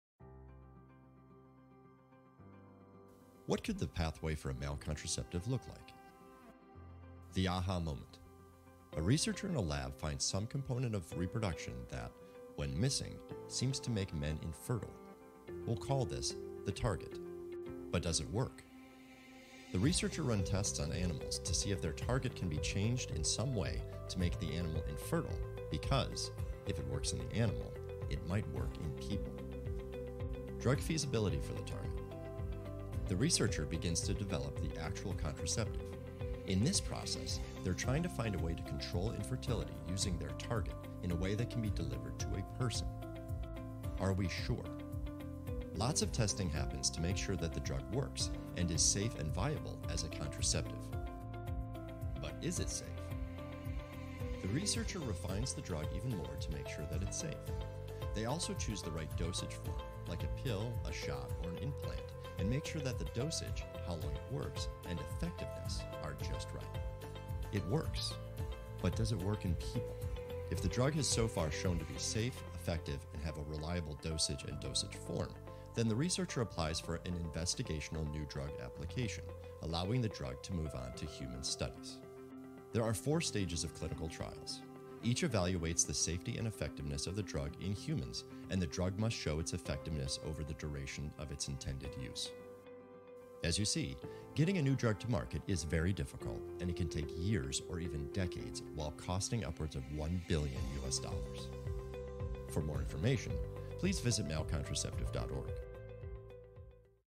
The Drug Development Pipeline
In this short animated film, we explain the drug development pipeline in an effort to illustrate why it takes so long and costs so much money to get a new contraceptive to market.

Visit us at malecontraceptive.org to learn more!